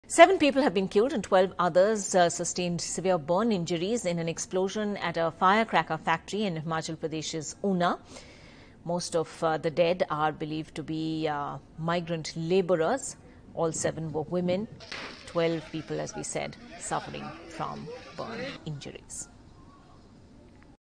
7 Dead In Major Explosion At Factory In Himachal Pradesh
Seven people were killed and 12 others sustained burn injuries on Tuesday in an explosion at a factory in Himachal Pradesh's Una district where firecrackers were being made, a senior district official said.